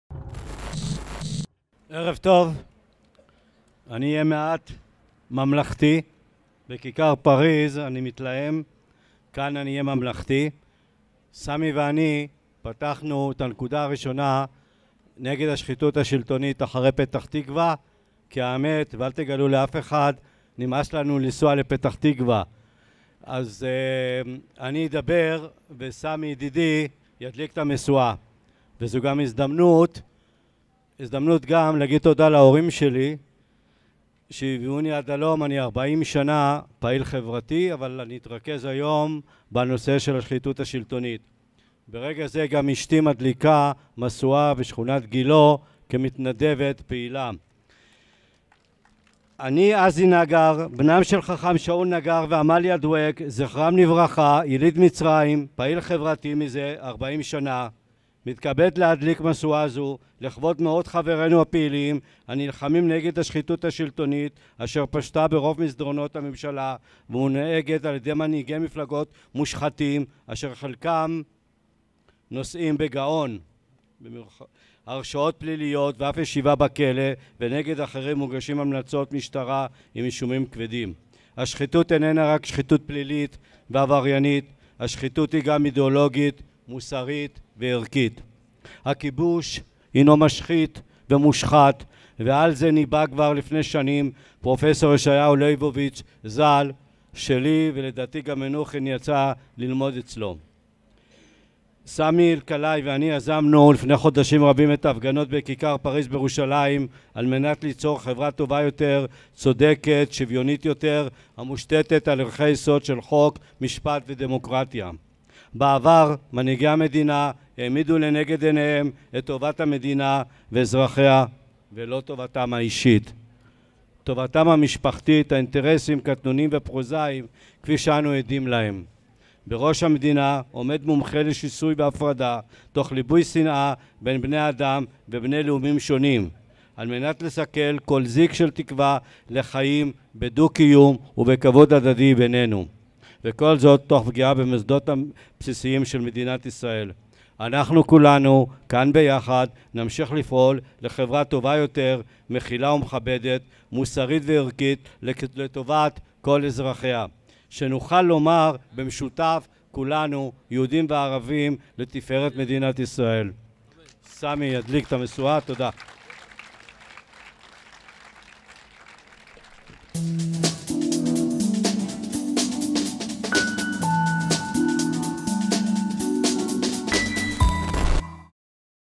עזי נגר וסמי אלקלעי - מדליקים משואה
עזי נגר וסמי אלקלעי, פעילים חברתיים נגד השחיתות, מדליקים משואה בטקס המשואות האלטרנטיבי של תנועת יש גבול, אפריל 2018.